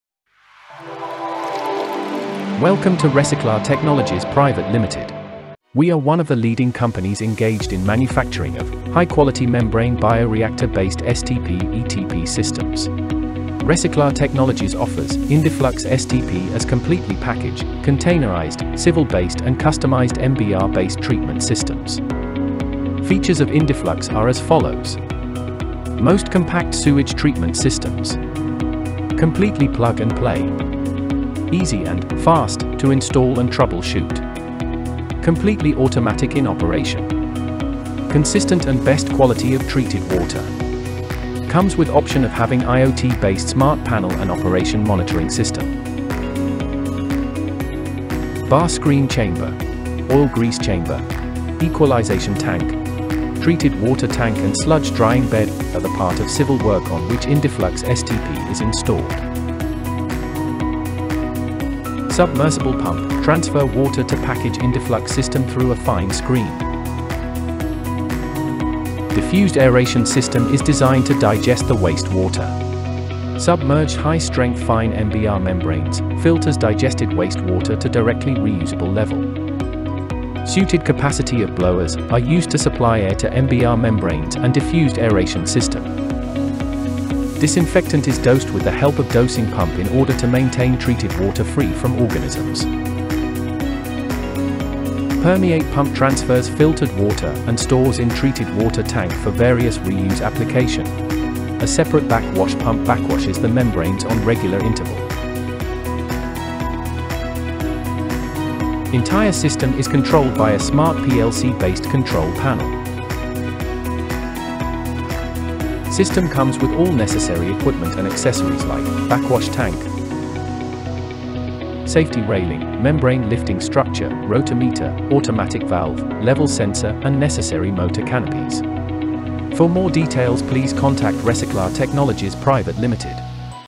INDIFLUX MBR STP
Indiflux - STP is one of the best quality of packaged / containerised - sewage treatment systems - used for Industries , Hotels, Hospitals, Apartment.  It comes with the most advance technology widely known as - Membrane Bio Reactor. It is automatic in operation and gives cosistent and high quality of treated water in output for the purpose of reuse